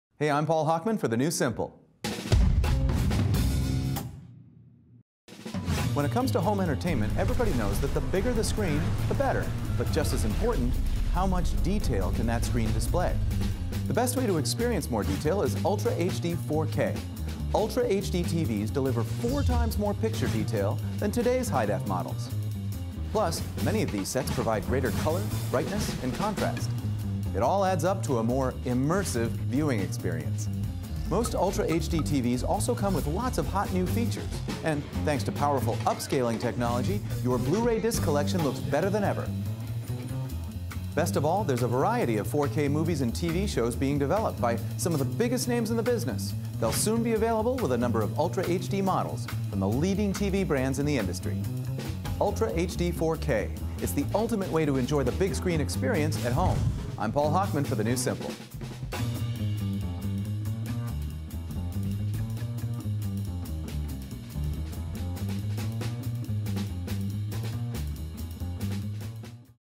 4K Ultra HDTV Promotional Video
4K Ultra High-Definition Television (Ultra HDTV) promotional video from the Consumer Electronics Association and DEG: The Digital Entertainment Group.